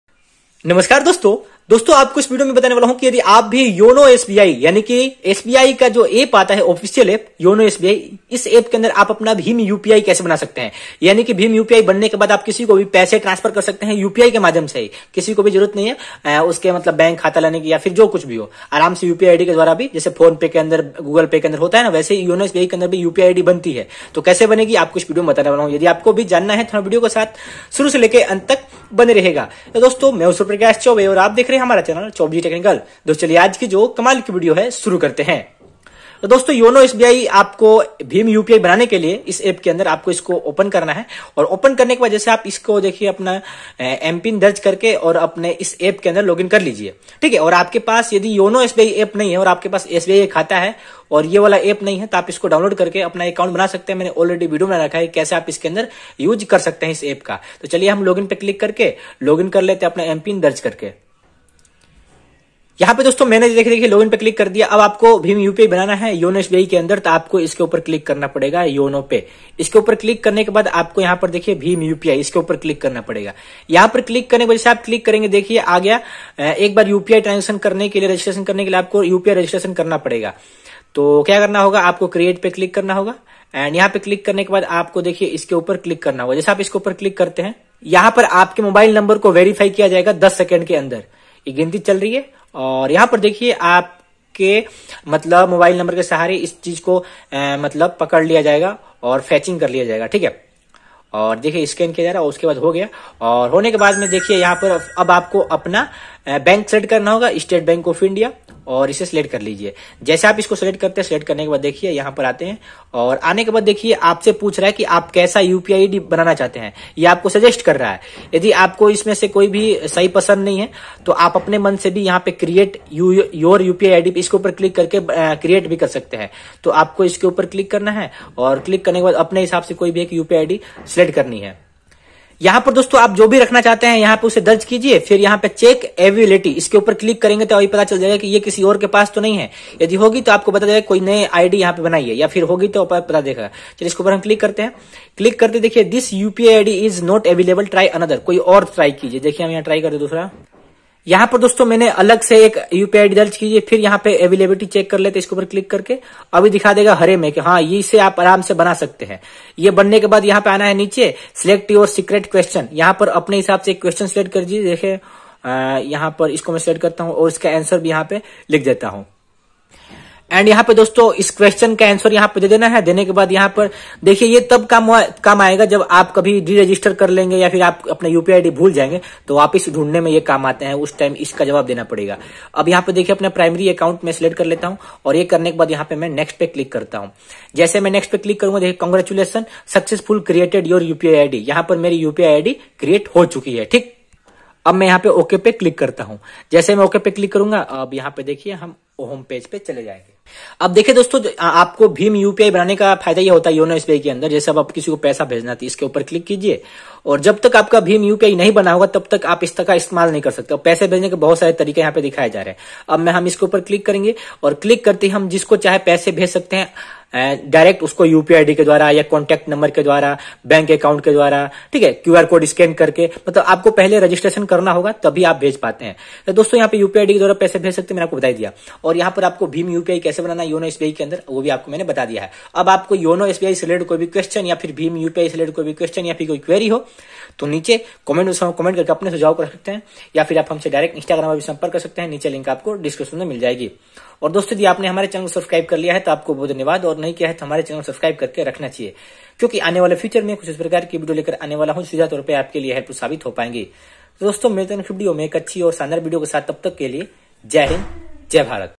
Yono sbi UPI registration kaise kare | yono sbi bhim upi kaise banaye | yono sbi upi id kaise banaye
Hello friend today I am describe that in this video how to register yono sbi bhim upi id.

Hero choubey